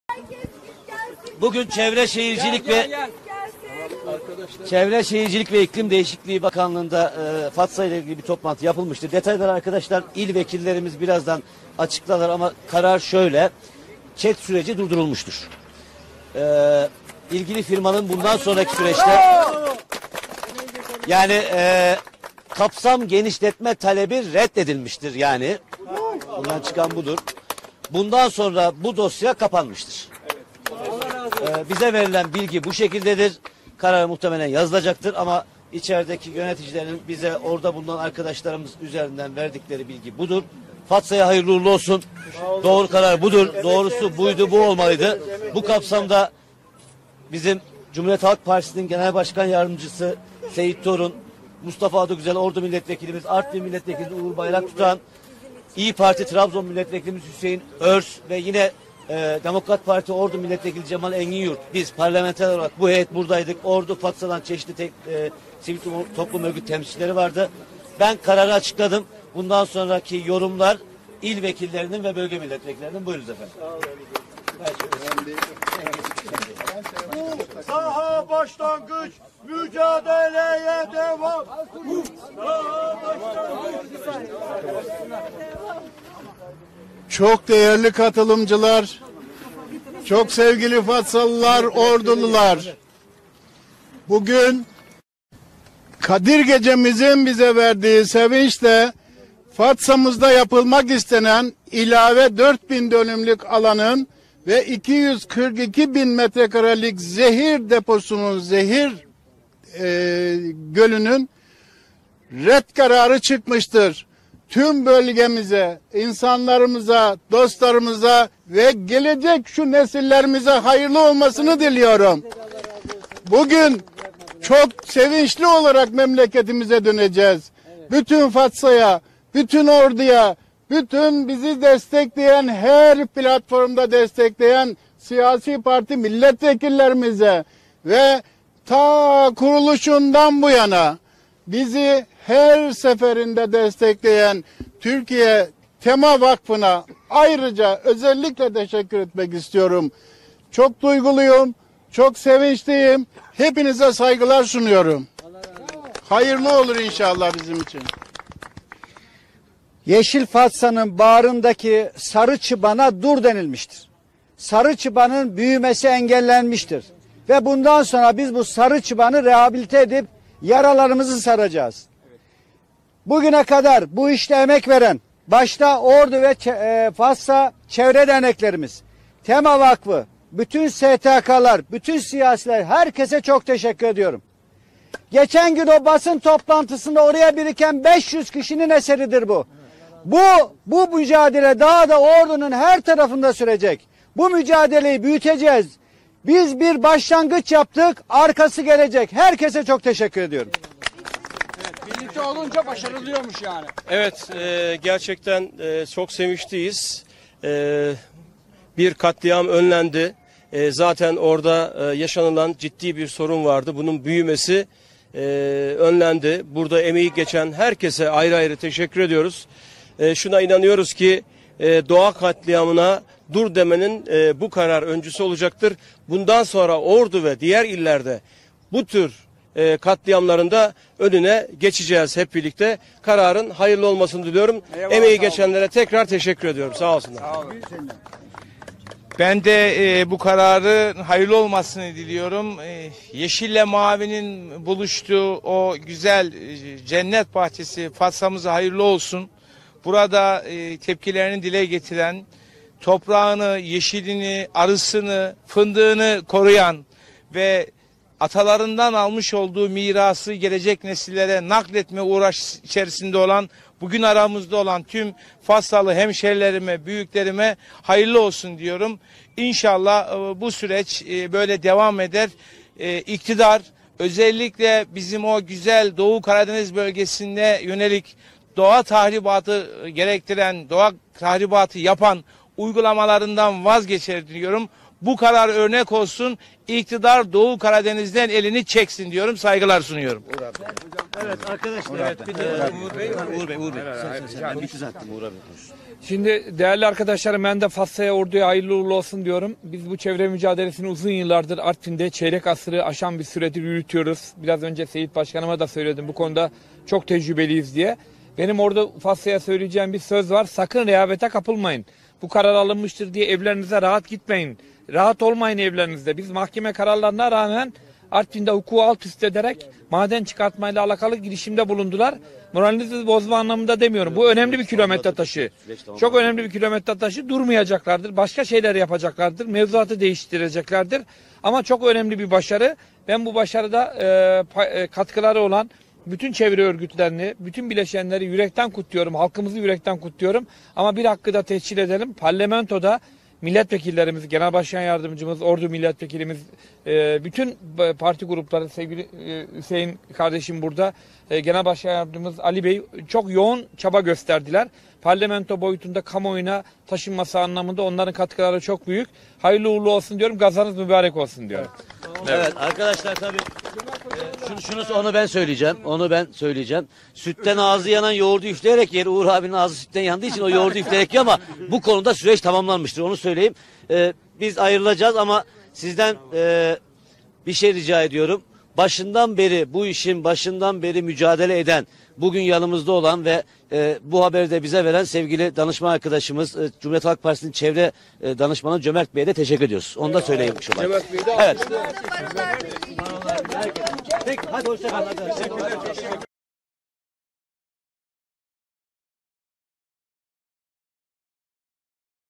Fatsa'daki Altın Madeni için yapılan ÇED başvurusu reddildi
Altıntepe Madencilik’in Ordu’nun Fatsa ilçesinde işlettiği altın madeninin kapasitesini artırmak ve bölgede siyanür gölü oluşturmak için yaptığı başvuru reddedildi. CHP Genel Başkan Yardımcısı Seyit Torun, “Bir katliam önlendi. Şuna inanıyoruz ki doğa katliamına ‘dur’ demenin, bu karar öncüsü olacaktır” dedi. CHP Ordu Milletvekili Mustafa Adıgüzel ise "Yeşil Fatsa’nın bağrındaki sarı çıbana ‘dur’ denilmiştir. Sarı çıbanın büyümesi engellenmiştir" diye konuştu.

İlkeli, bağımsız, objektif haber...